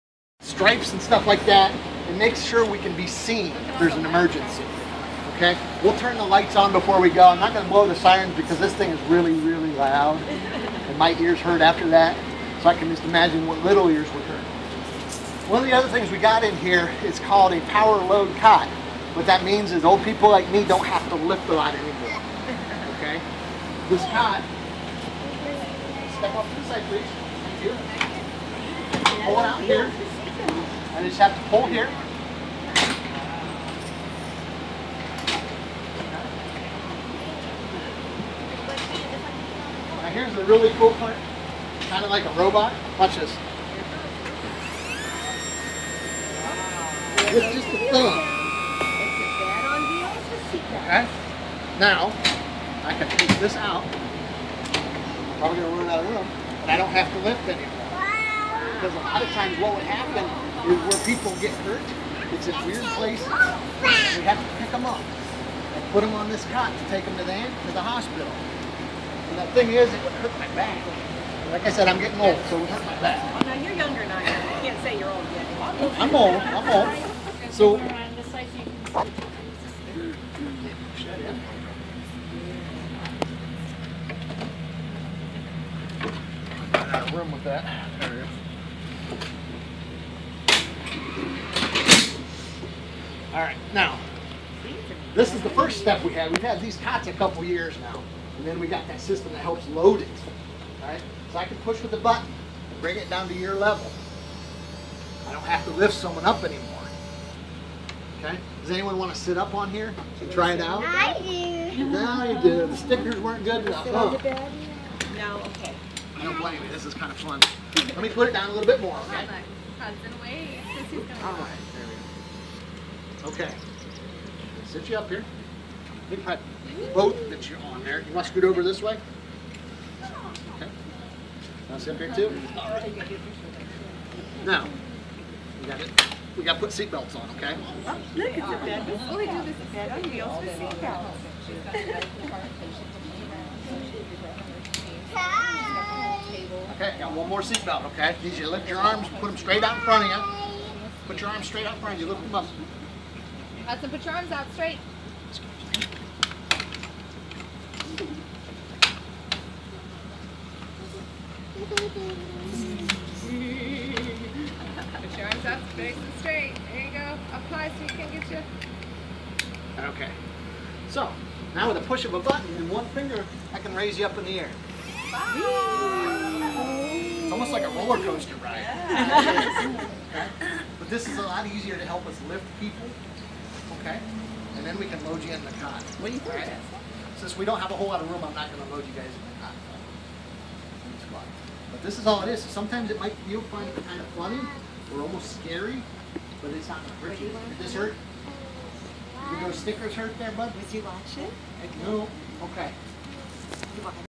Rittman EMS chief shows off new ambulance
Rittman EMS Chief Andy Baillis was the featured Storytime speaker at the Rittman Branch of the Wayne County Public Library. He showed kids the city's new ambulance, and he let them check it out inside.